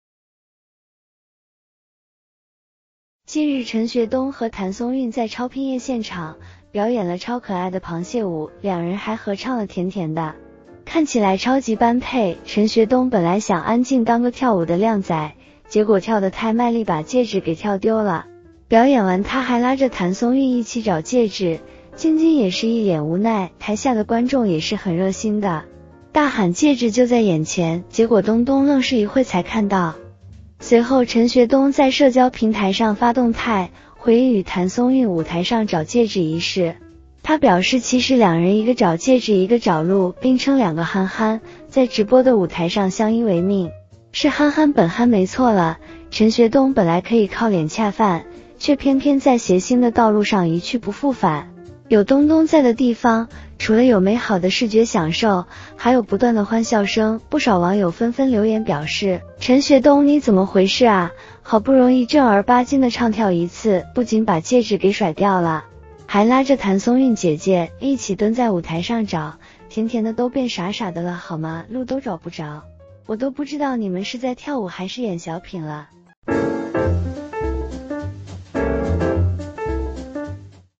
陈学冬谭松韵舞台上找戒指，一个趴地上另一个找不到路，太憨憨了
陈学冬和谭松韵表演了超可爱的螃蟹舞，冬冬戒指跳完舞不见了，表演完他还拉着晶晶满舞台的找，这俩人也太好玩儿了。